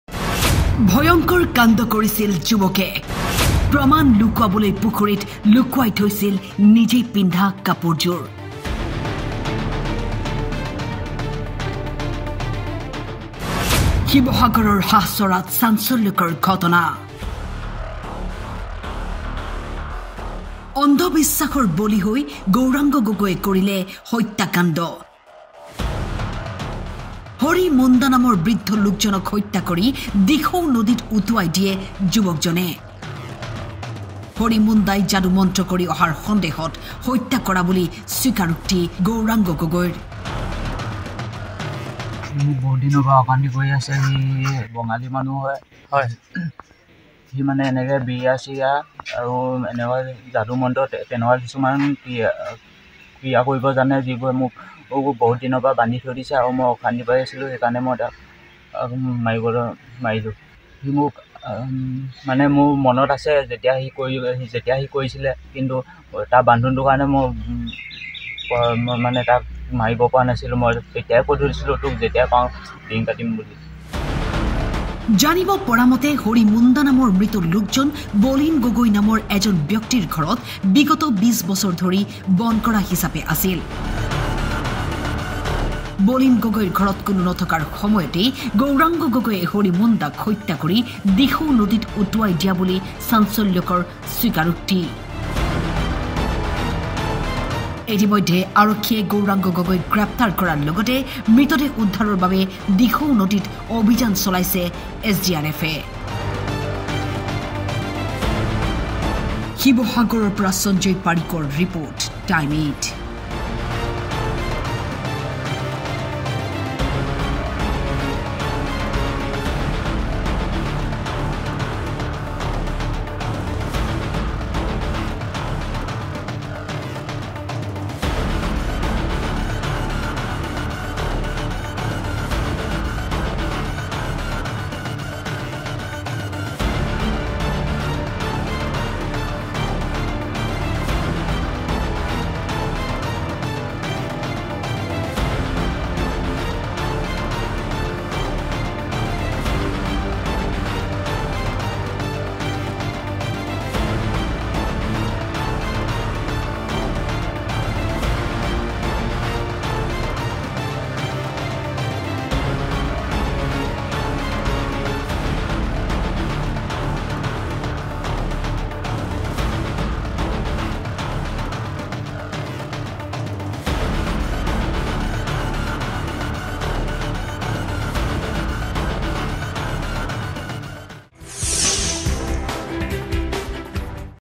শিৱসাগৰৰ হাঁহচৰাত চাঞ্চল্য়! যুৱকে কৰিলে ভয়ংকৰ কাণ্ড!
শিৱসাগৰৰ হাঁহচৰাত চাঞ্চল্য়! যুৱকে কৰিলে ভয়ংকৰ কাণ্ড! পুখুৰীত লুকুৱাই থৈছিল প্ৰমাণ...